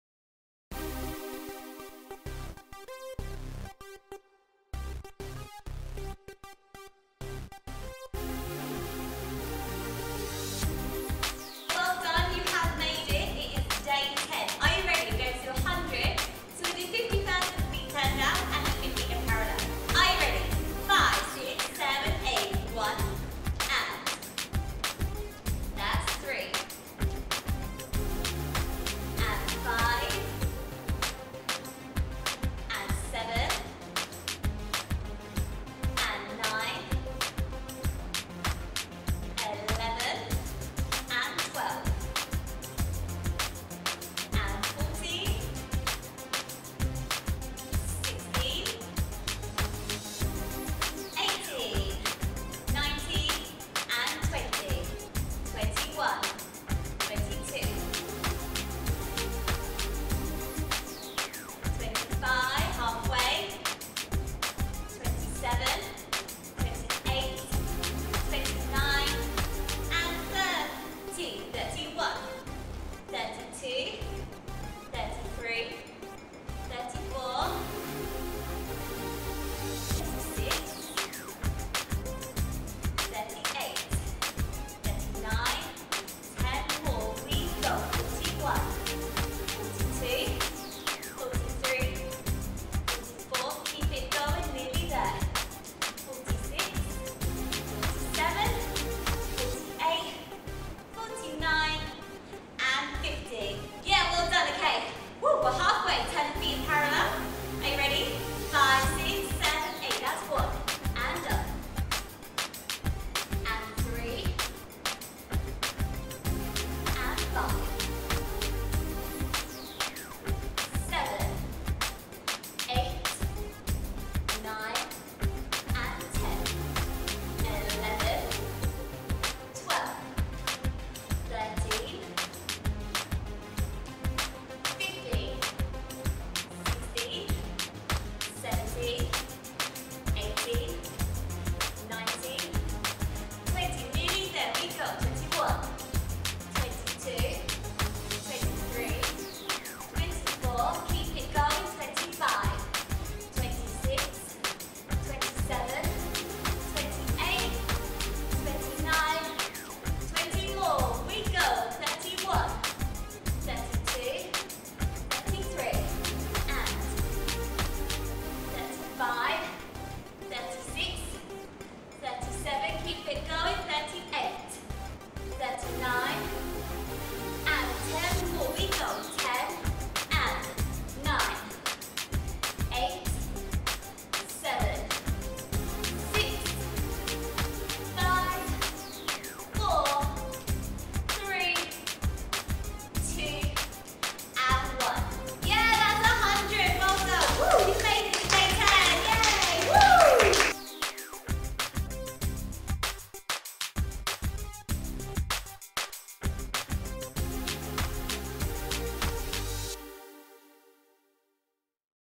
100 Squat Challenge Day 10 (100 squats) Fitness Workout Dance 2 Enhance Fitness
It's Day 10 well done you have made it to the last day of the 100 squat challenge, 100 squats left and then you have completed the challenge, a big well done to everyone who has joined in.

If you have completed every day you would have done 550 squats which is amazing.

Do you have any suggestions for what you would like to see for the next challenge, we are going to do a 10 minute Hula Hoop challenge, if you have any other good ideas please comment and let us know.

The D2E Fitness Team